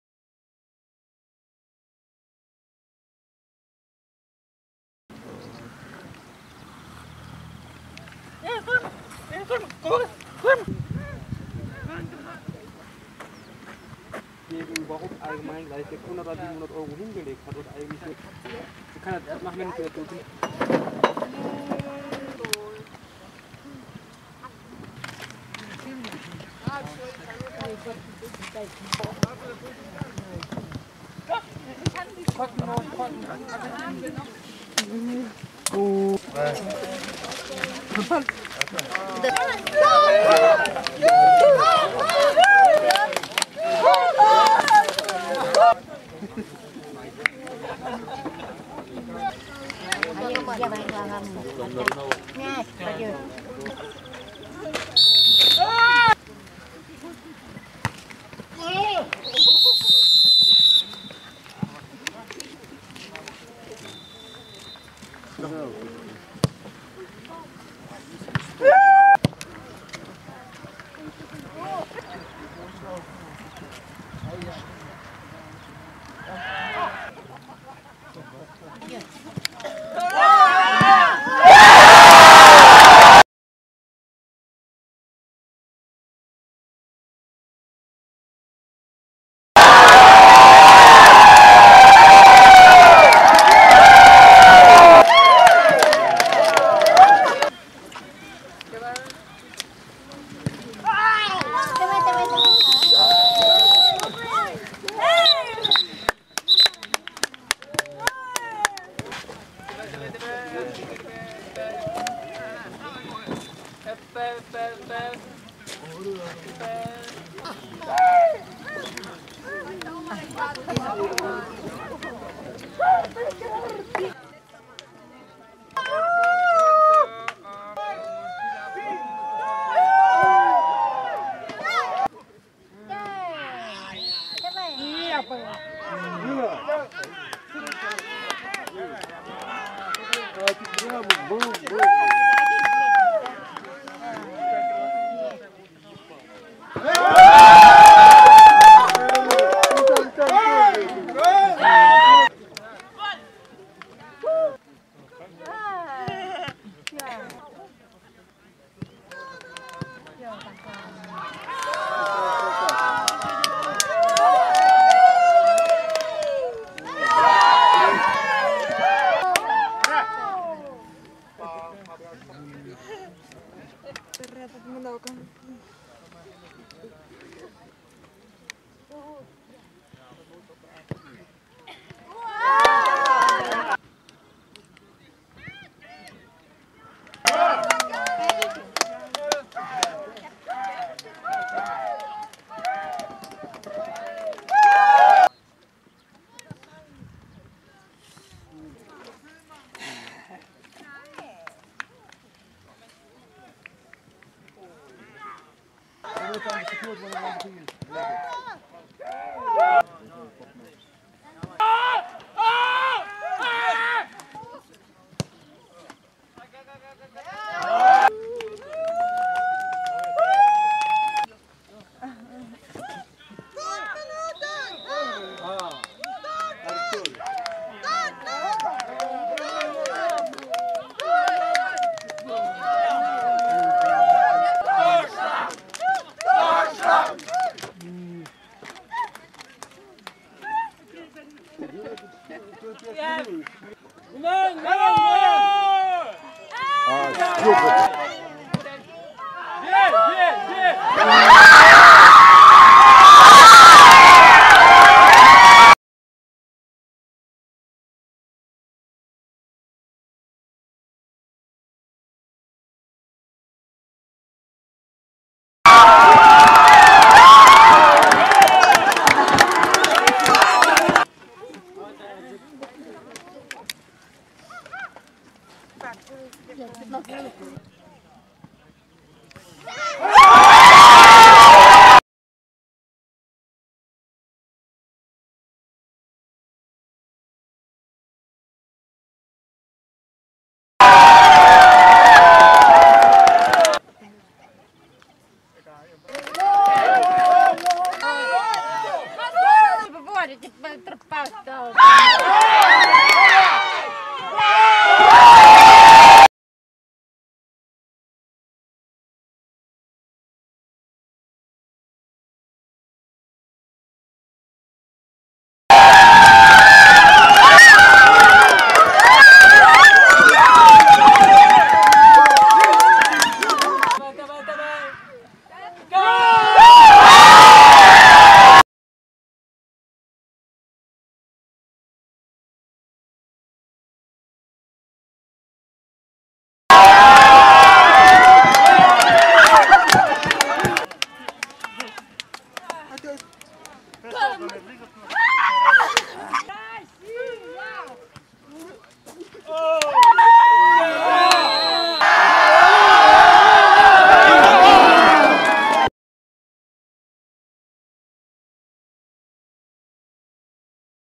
Deaflympics13: Semifinale Tyskland - Rusland
Deafbold udsendelse udgivet på YouTube d. 3. august 2013 fra Deaflympics 2013 i Bulgarien.

- Kampudsendelse SEMIFINALE Tyskland - Rusland 1-4 (1-1) Forlænget spilletid